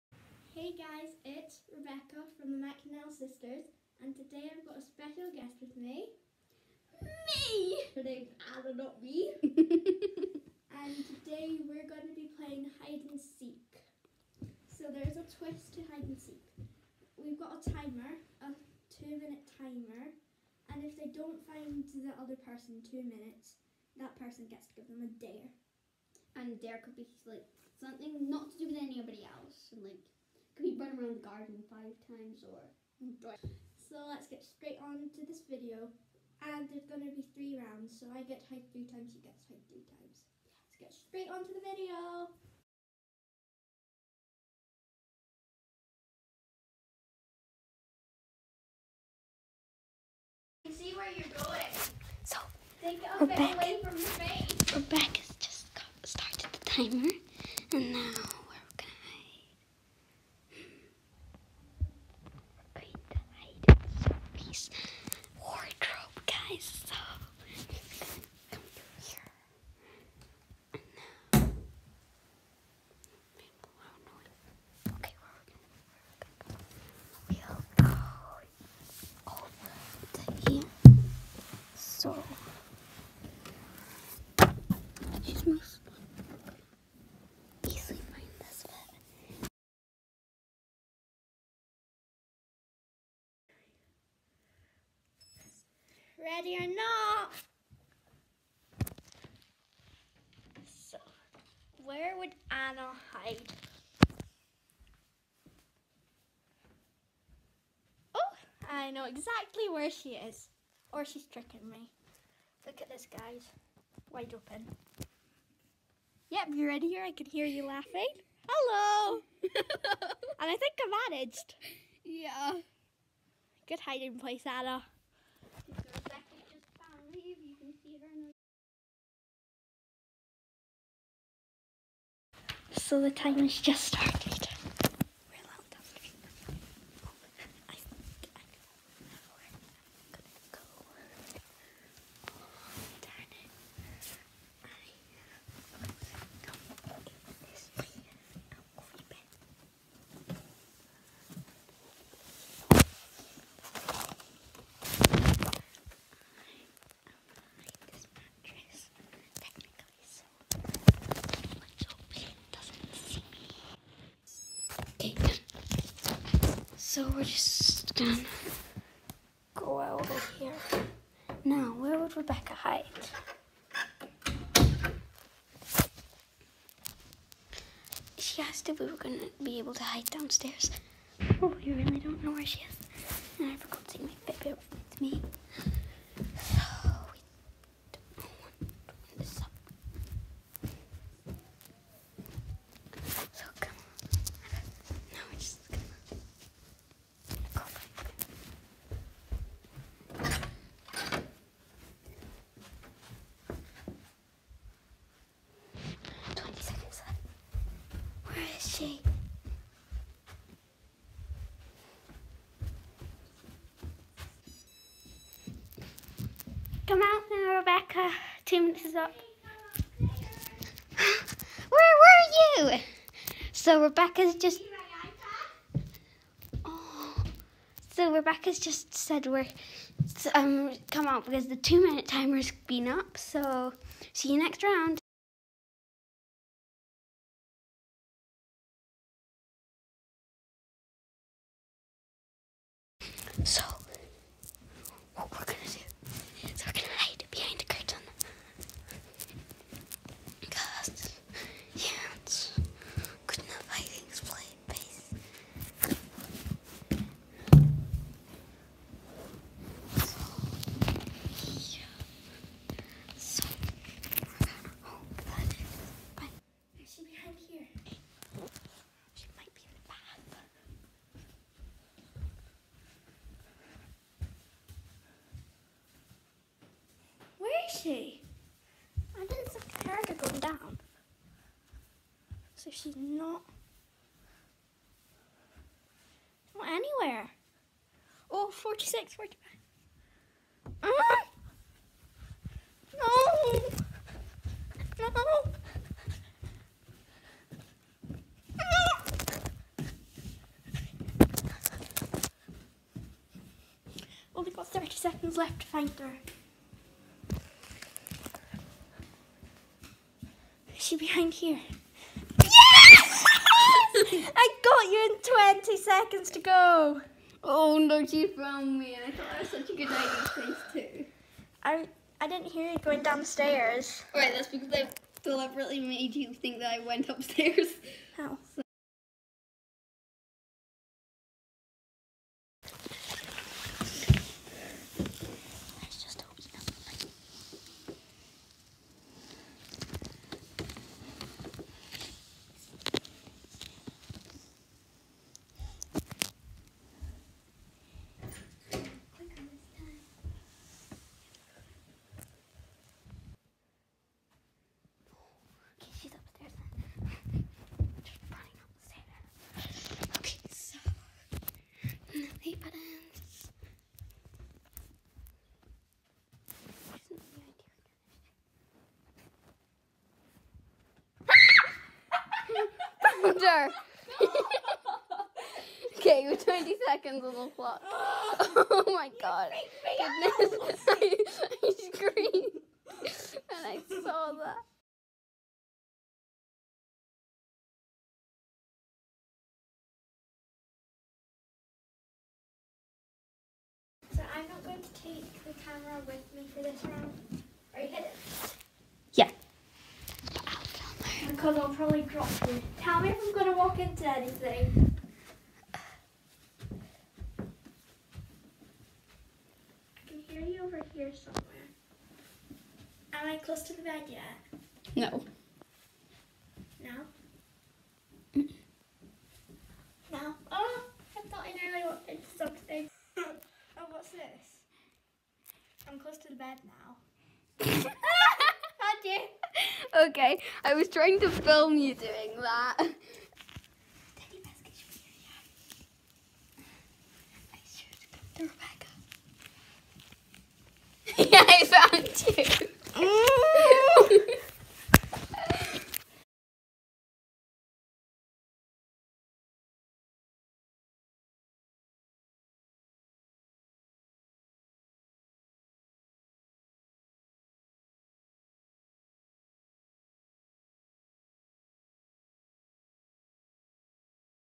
Hide & Seek
Rebecca from the McDonell Sisters with Anna play Hide & Seek